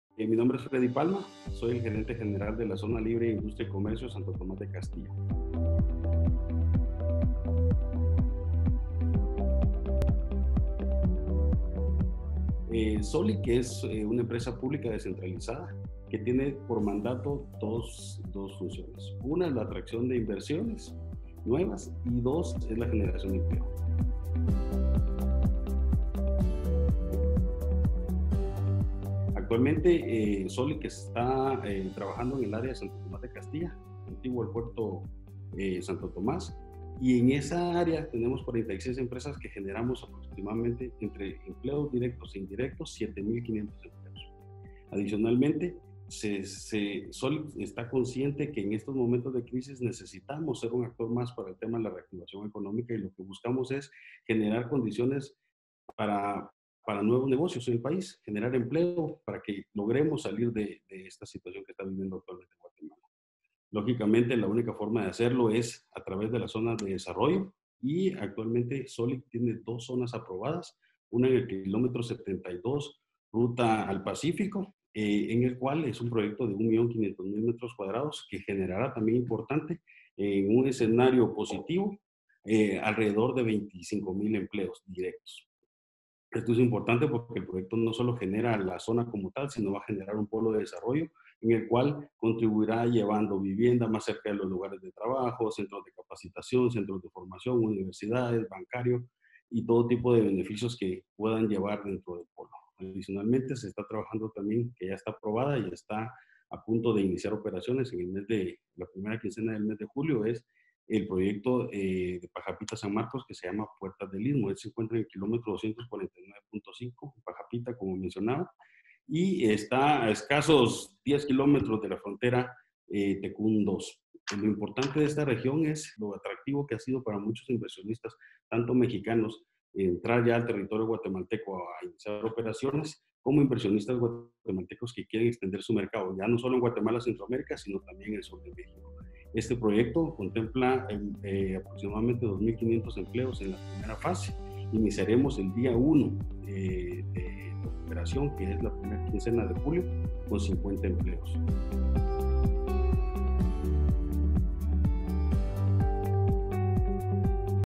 Nuevas zonas francas impulsarán la economía
Fredy Palma, gerente general de la Zona Libre de Industria y Comercio “Santo Tomás  de Castilla” explica que seis Zonas de Desarrollo Económico Especial Públicas (ZDEEP) se impulsan tanto por el Estado como por empresas privadas para la generación de empleo y atracción de industrias.